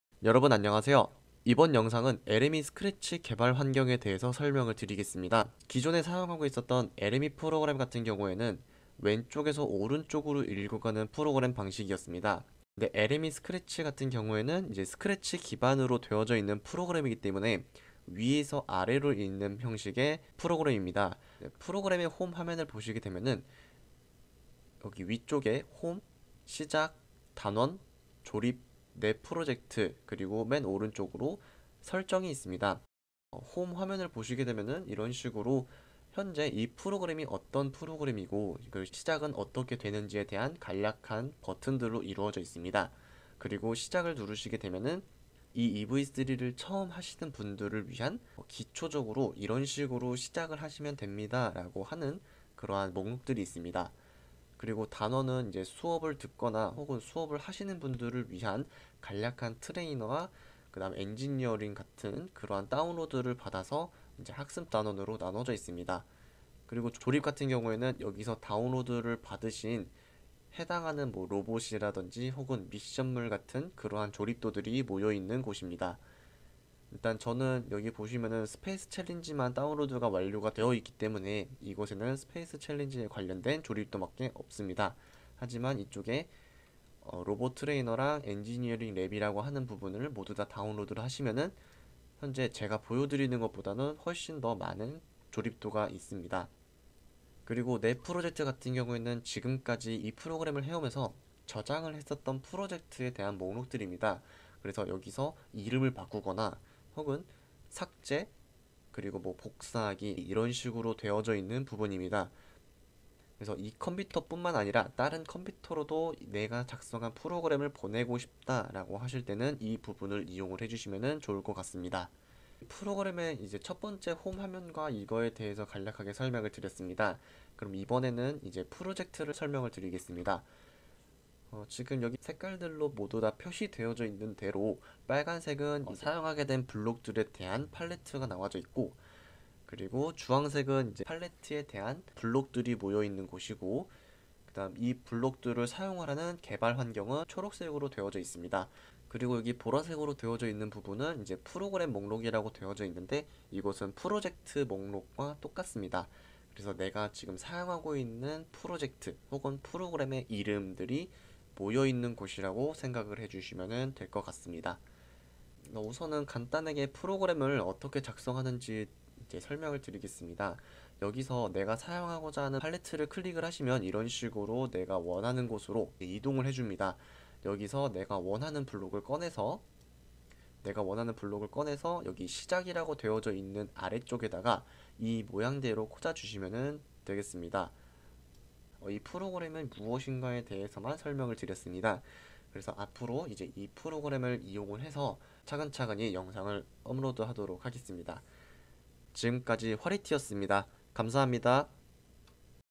[EV3 코딩] - LME Scratch 개발환경 간략 설명 - 화리트(Hwarite)
기존 LME와 LME Scratch 프로그램의 어떤 차이가 있는지와 프로그램 인터페이스의 대한 간략한 설명 영상입니다.

00:00 인사, 영상소개
00:05 LME와 LME Scratch 간단 비교 설명
00:22 홈 화면 설명
02:10 프로그램 개발 환경 설명